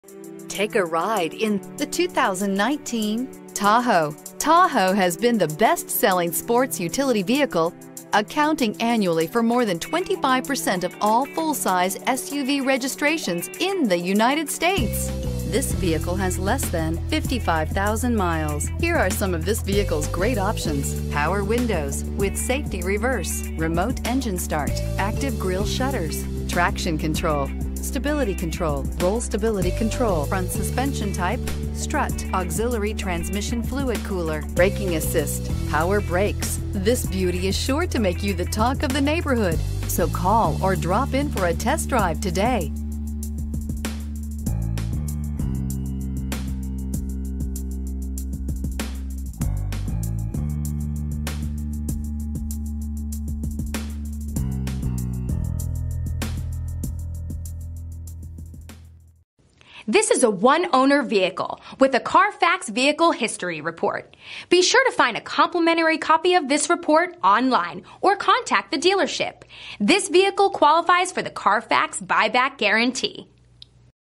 2019 Chevrolet Tahoe Houston TX R350700A
2019 Chevrolet Tahoe LT

Make sure to get your hands on this 2019 Chevrolet Tahoe LT with a backup sensor, rear air conditioning, remote starter, backup camera, parking assistance, braking assist, dual climate control, hill start assist, stability control, and traction control before it's too late!  It comes with a 8 Cylinder engine.  This SUV is one of the safest you could buy. It earned a crash test rating of 4 out of 5 stars.  For a good-looking vehicle from the inside out, this SUV features a stunning black exterior along with a jet black interior.  Don't wait on this one.